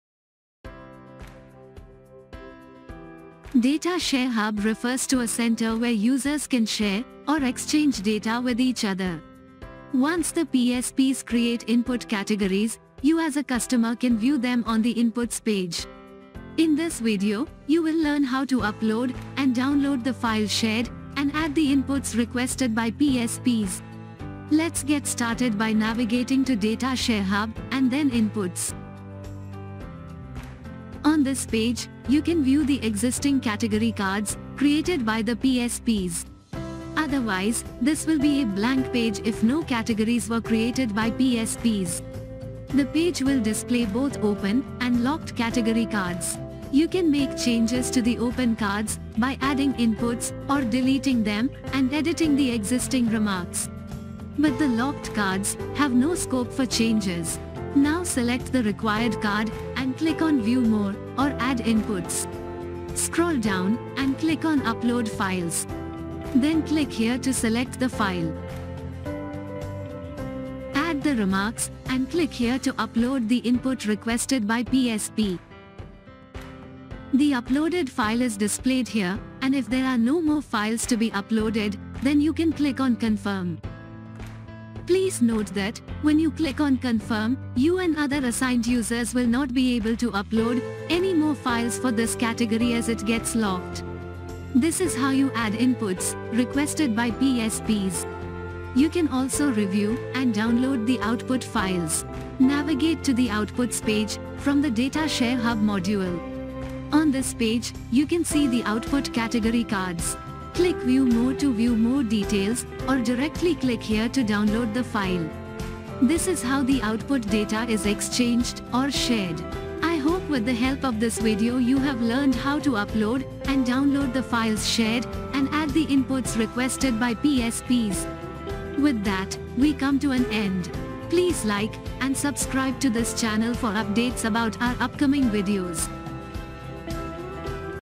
Upload and Download Files in DataShare Hub on greytHR
greytHR's Datashare hub is a centralized platform that enables users to exchange and share data with ease. By creating input categories, PSPs can provide customers with clear visibility of their data options on the Inputs page, simplifying the process of data sharing and collaboration.